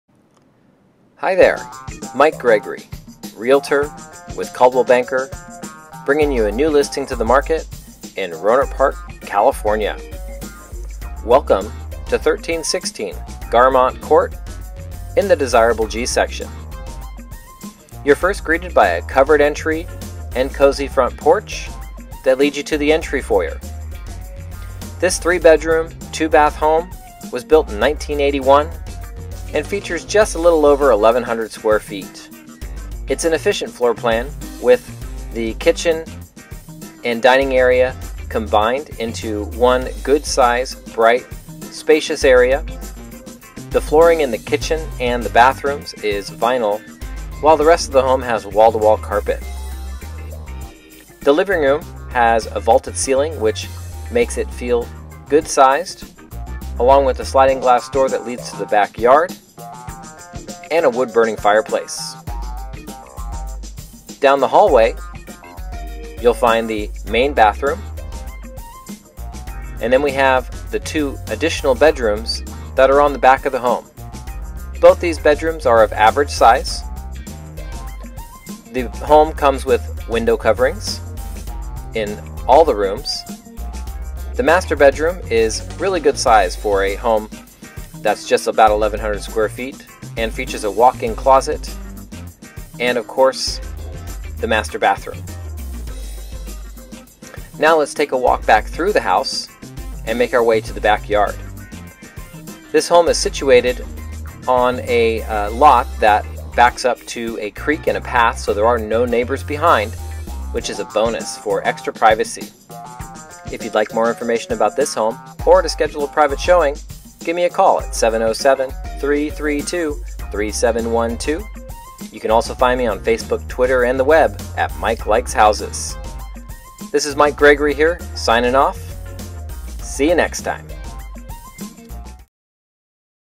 1316 Garmont Court, Rohnert Park CA
New listing in Rohnert Park's desirable G Section!  Single level 3 bedroom, 2 full bath home built in 1981 with just over 1100 square feet of private lot that backs to a path and creek.  Well Priced at $375,000.